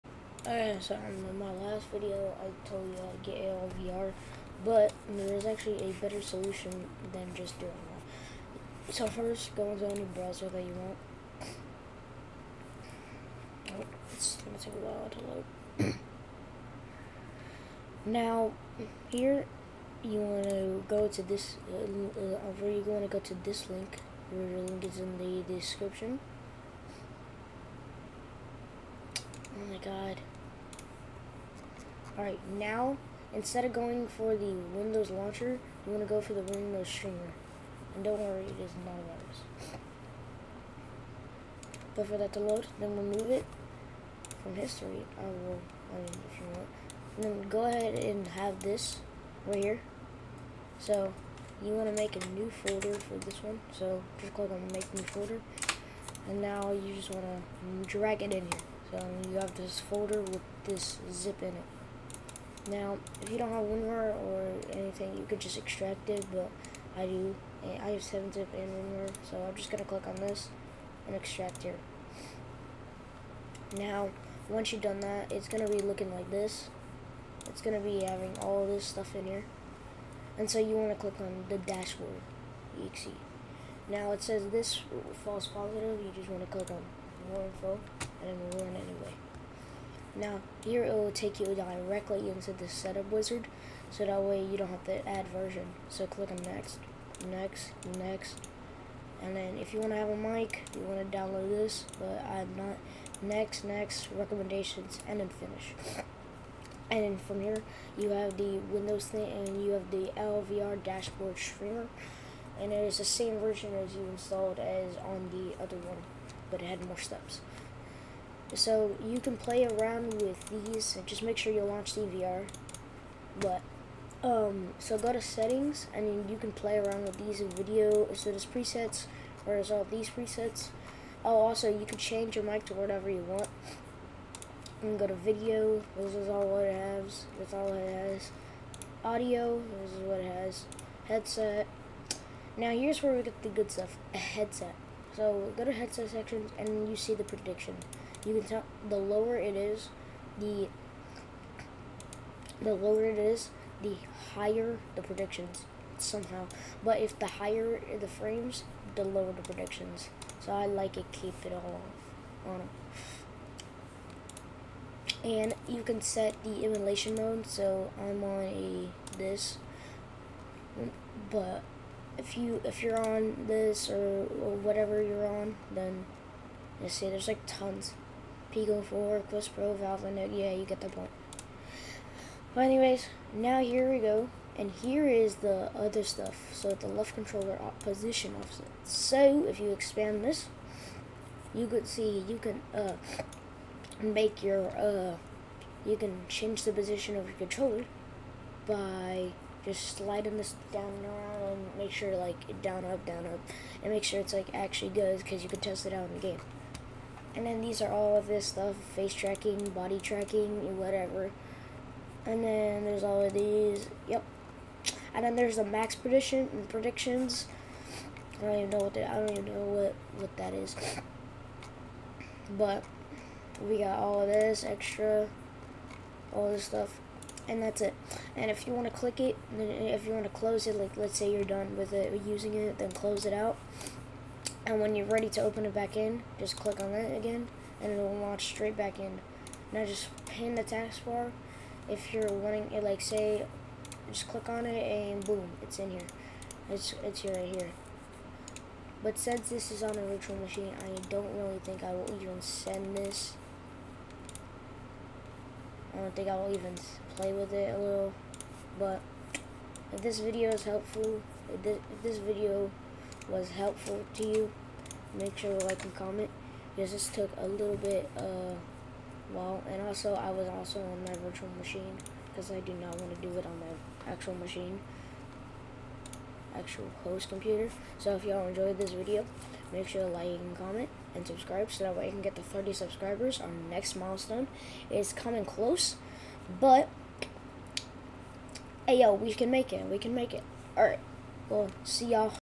Updated tutorial on how to get ALVR
In this video i show you how to get ALVR which is a software designed to gorilla tag for settings and prediction and position offsets! This is also a updated tutorial since that other one had more longer steps than this one!